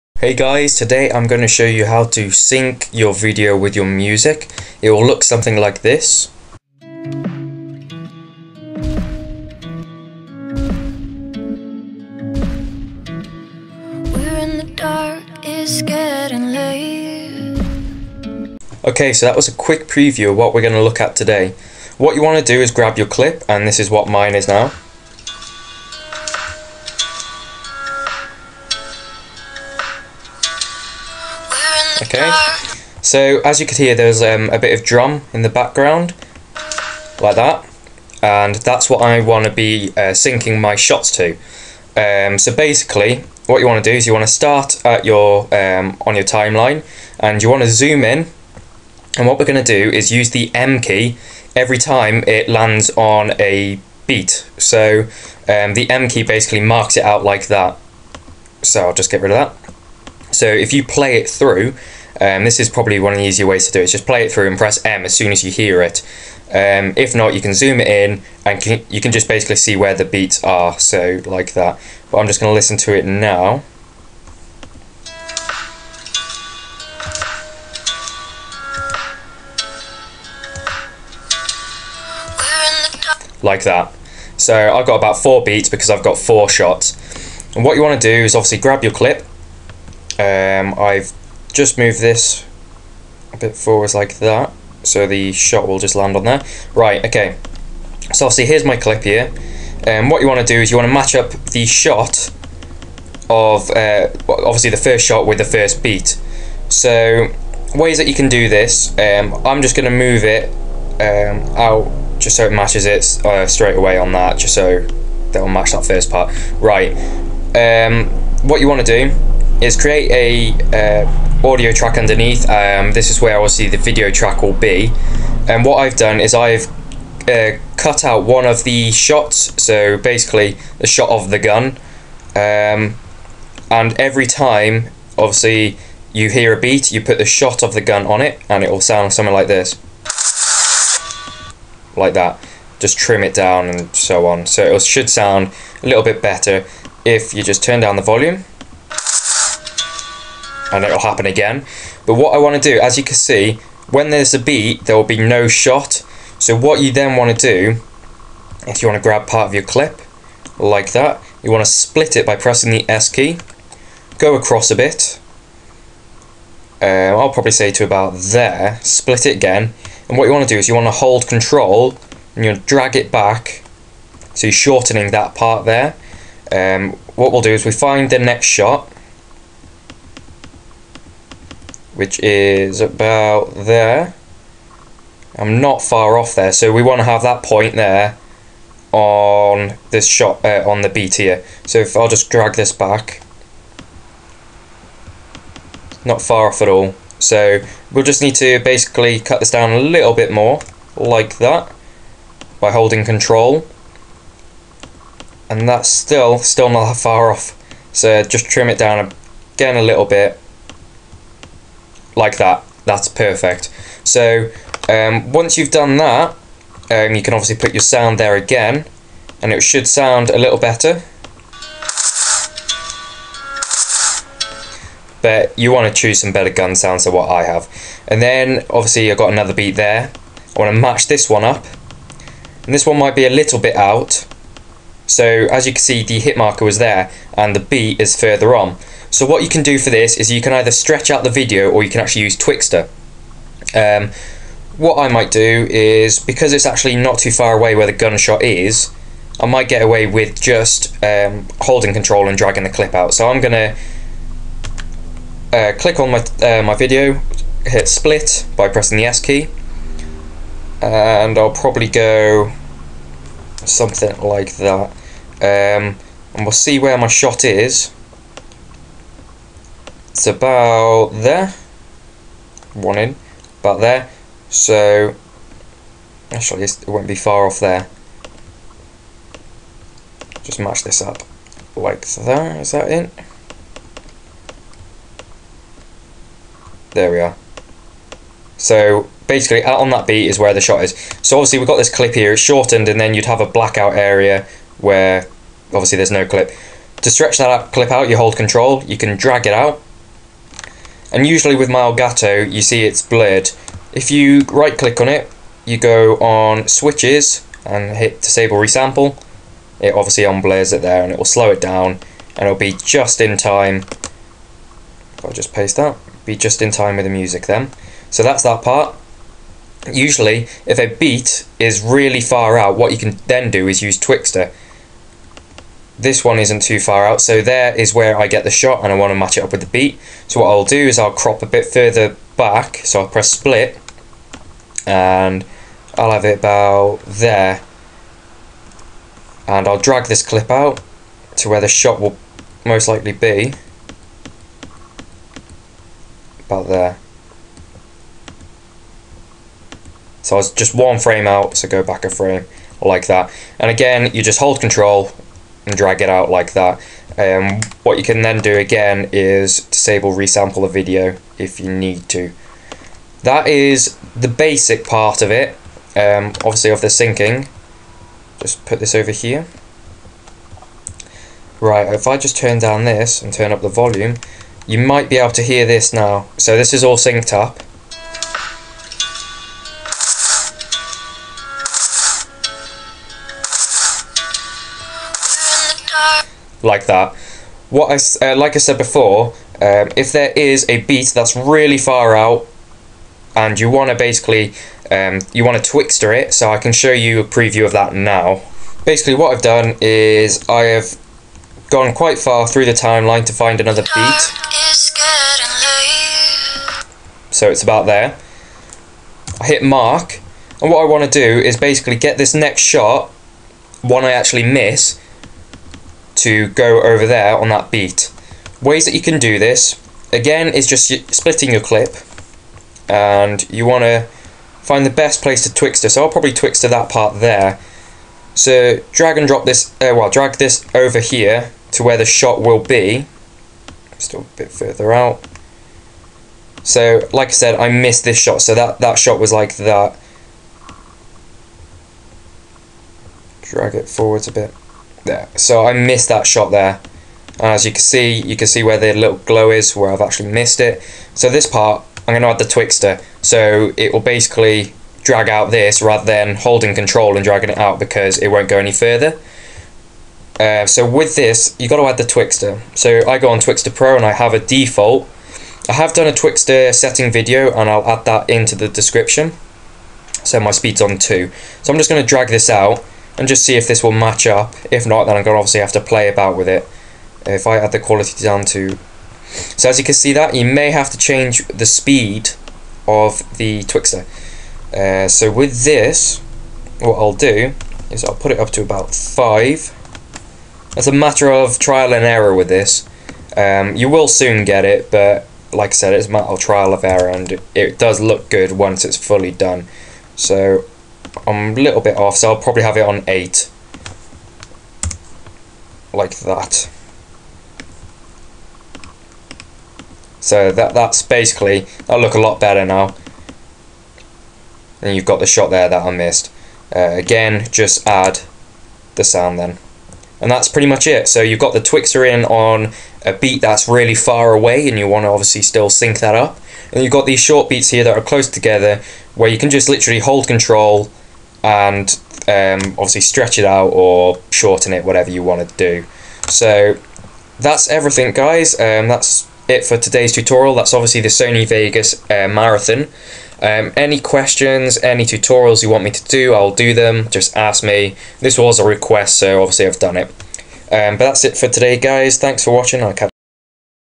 Sony Vegas Tutorial- Syncing with music (Marathon 1)
READ: This is Part 7 of my sony vegas montage effects editing series. its a tutorial on syncing gun shots with music